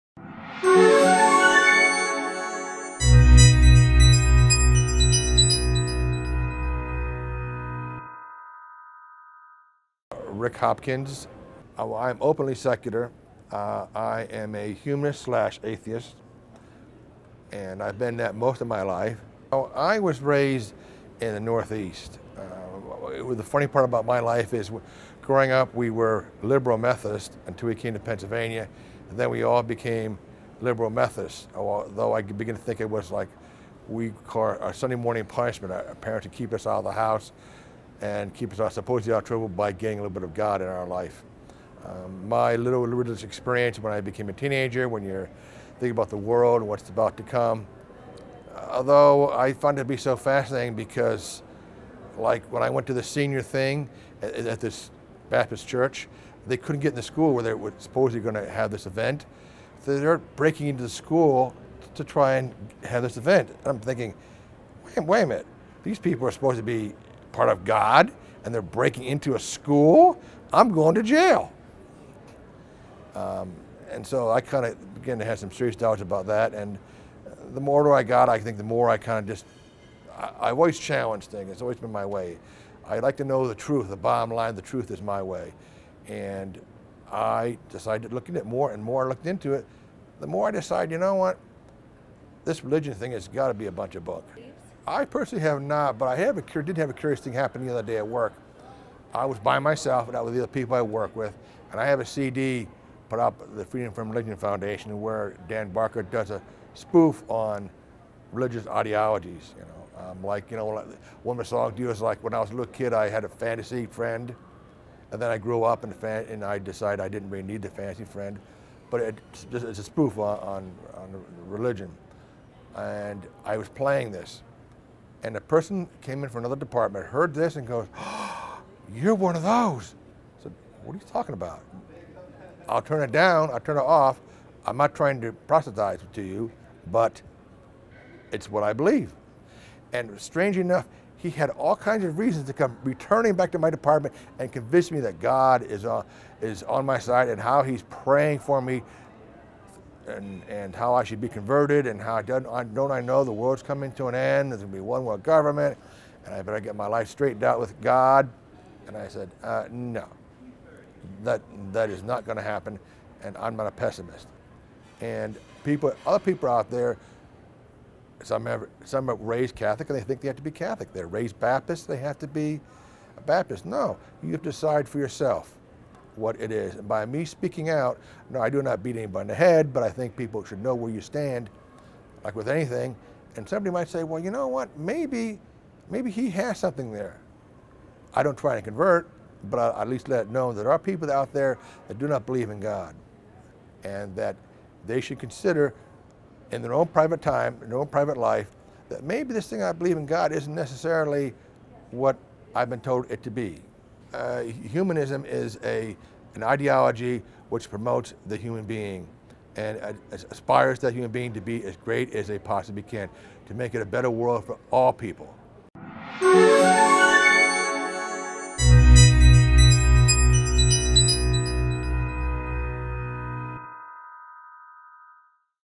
Ric Hopkins - Openly Secular
Ric is a humanist/atheist and is Openly Secular! He was born in a religious household, and began to have doubts about religion when he encountered religious people who were doing questionable acts.Ric thinks that everyone needs to decide for themselves what they believe.